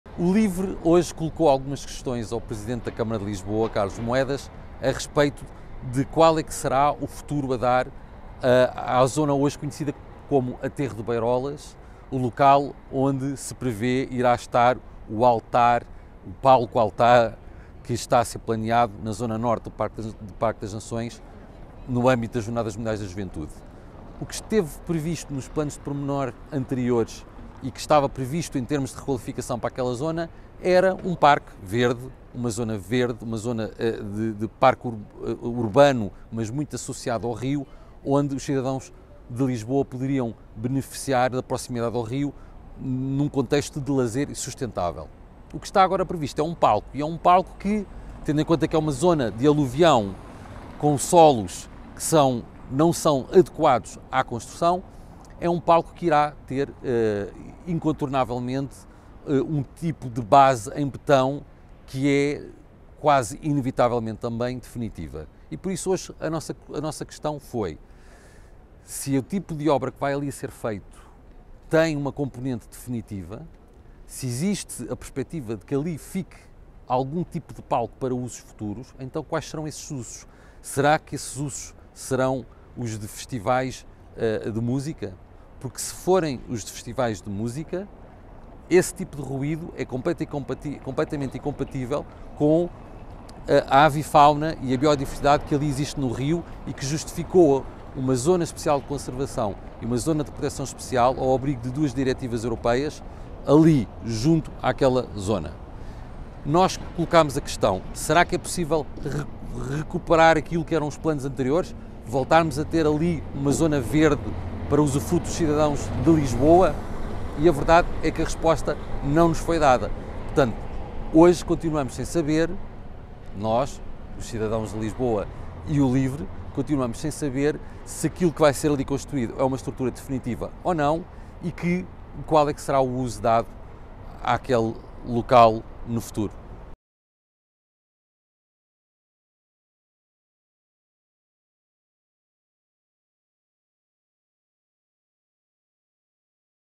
Qual o impacte ambiental do palco das Jornadas Mundiais da Juventude?
Há questões sobre o palco que ainda estão por responder. Foi estudado o impacte ambiental numa zona de grande biodiversidade no Tejo? A que fins se destina a infraestrutura? O palco será permanente ou não? Carlos Teixeira não recebeu respostas de Carlos Moedas na reunião da CML.